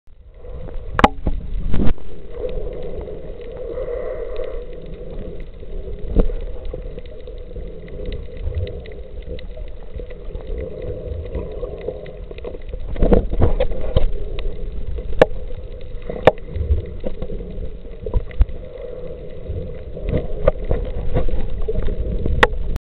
Sea Cucumber.AVI
Magen's Bay, St. Thomas, US Virgin Islands. An underwater video of a sea cucumber, a living creature of some variety taken by Barb W. while snorkeling the pristine emerald waters of the Carribean.